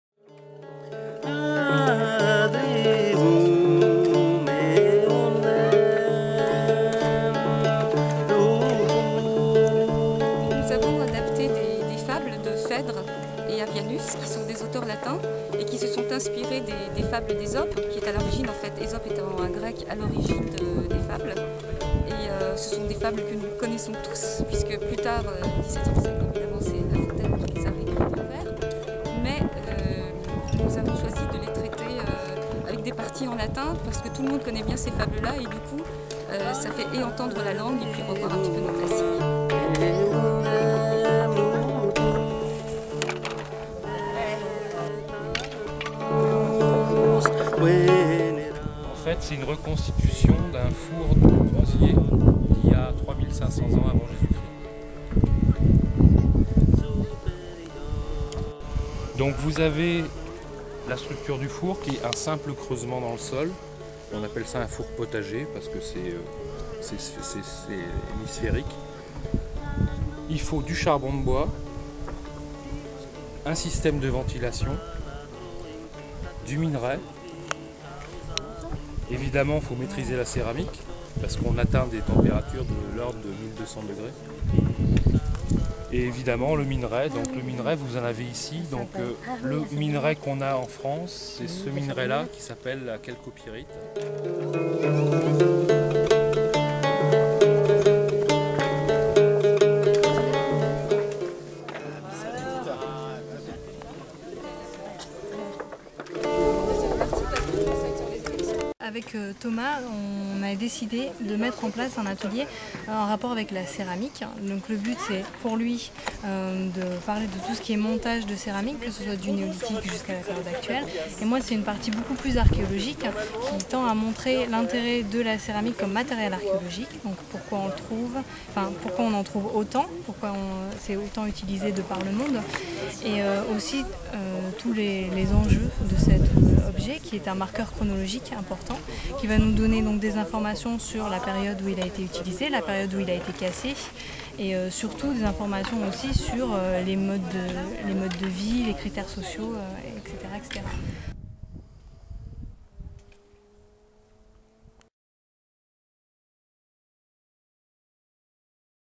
Naintré: journée antique au Vieux-Poitiers
Le site du Vieux-Poitiers à Naintré organisait dimanche 24 juillet sa septième journée antique. Jeux, spectacle et ateliers étaient au programme.